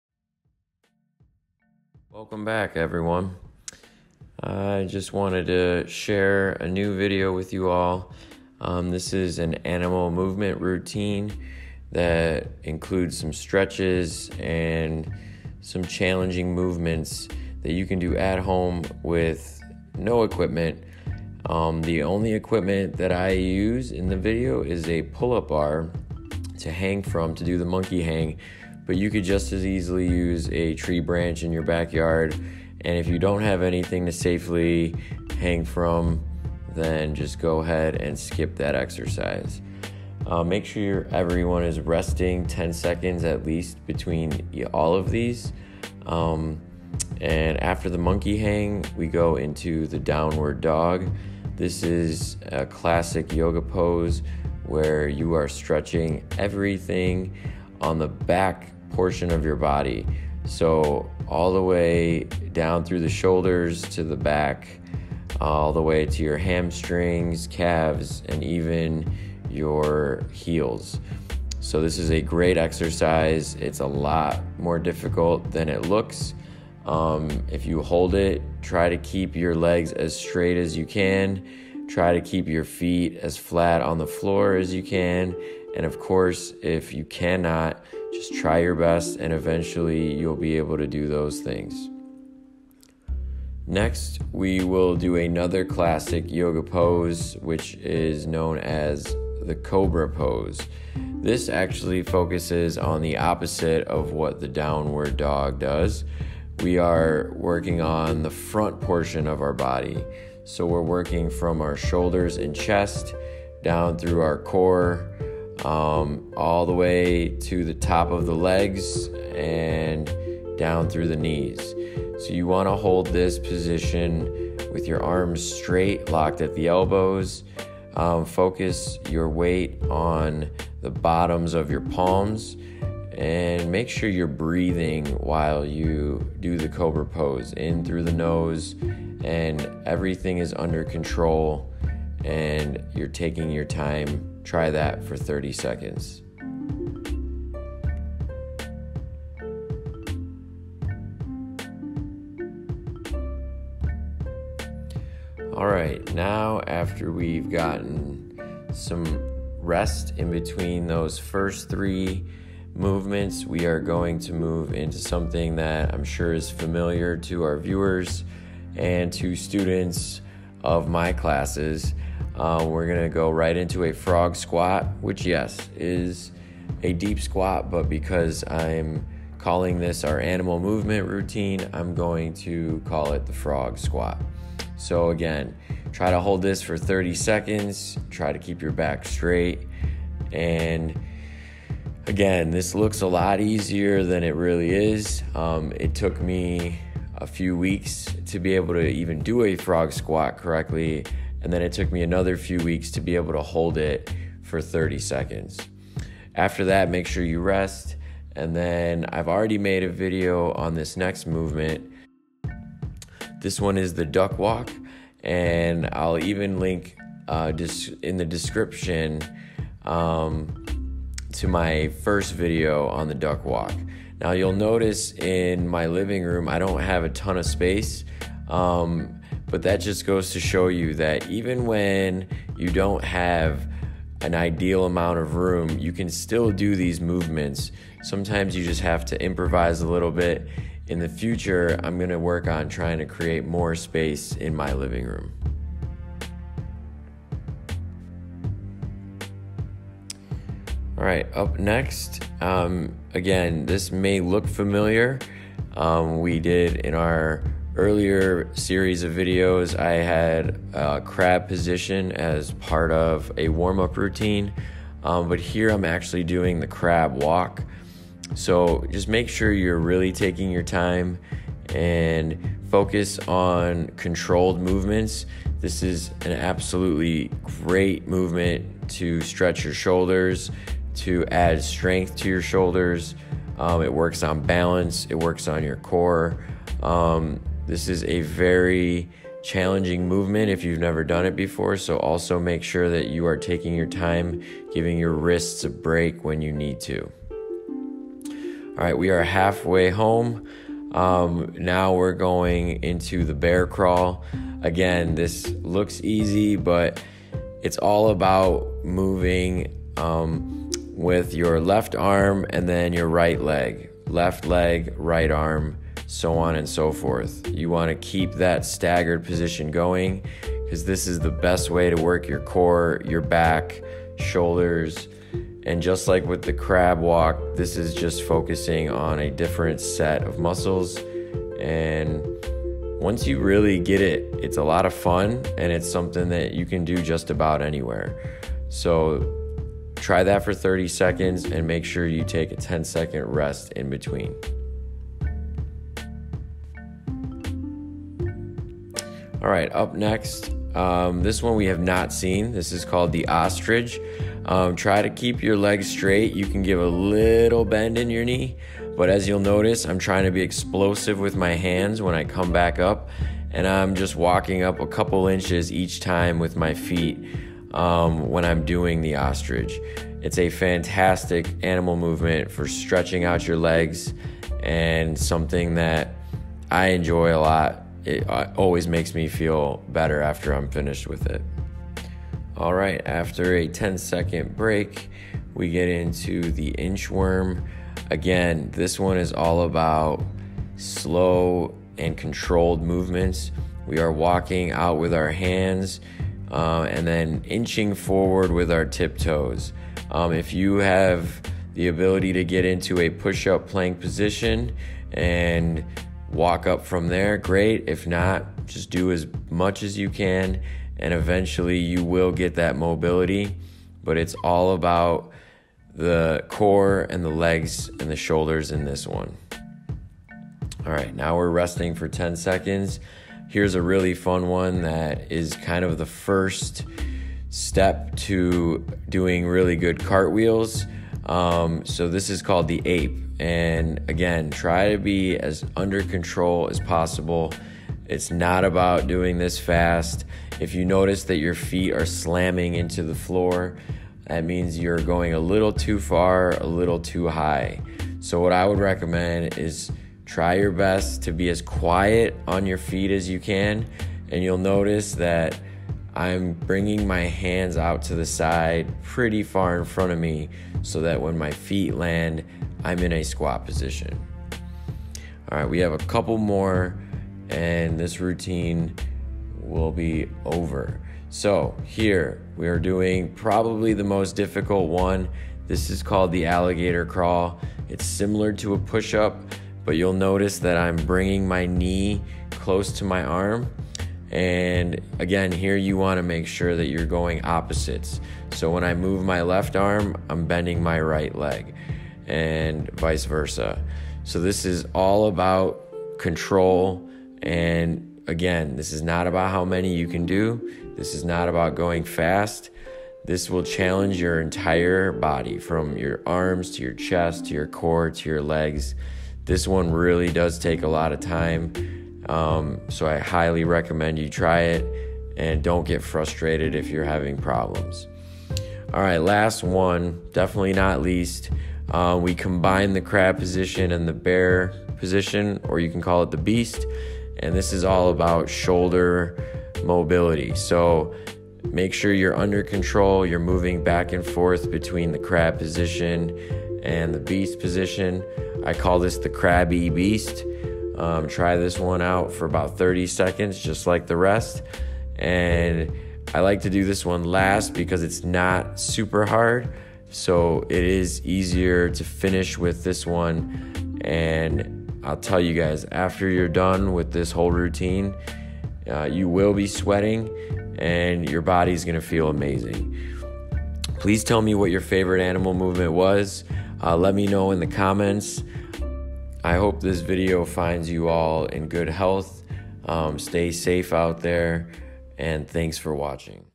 Physical Education at Home - Animal Movement Routine
Please follow along and join me in this animal movement work out that is sure to challenge you! In this video we have some stretches, some strength training and some different movements that you may have never done before. Please remember to practice this routine safely. If some of these movements are too difficult just stick with them and they will get easier after a few weeks.